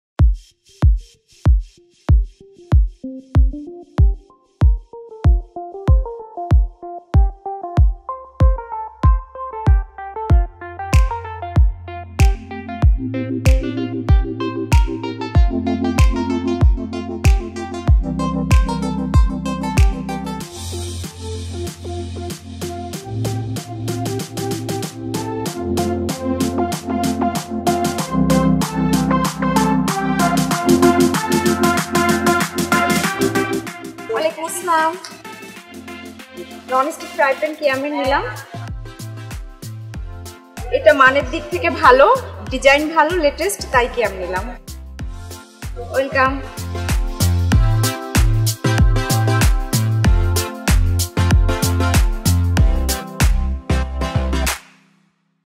'কিয়াম প্রোডাক্ট এক্সপেরিয়েন্স’ পর্ব ৭
ঢাকা নিউ মার্কেটে 'কিয়াম প্রোডাক্ট এক্সপেরিয়েন্স’-এ সম্মানিত গ্রাহক জানালেন কেন কিয়াম প্রোডাক্ট তার পছন্দ।